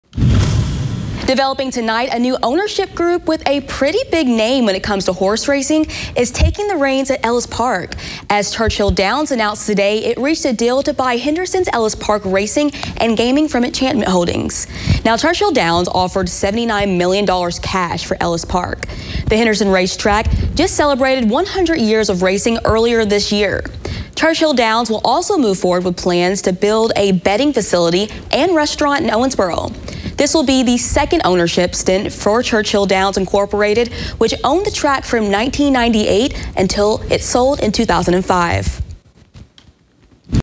Ellis Park in Henderson to be sold to Churchill Downs for $79 million
Churchill Downs Incorporated made the announcement of the deal to purchase Ellis Park.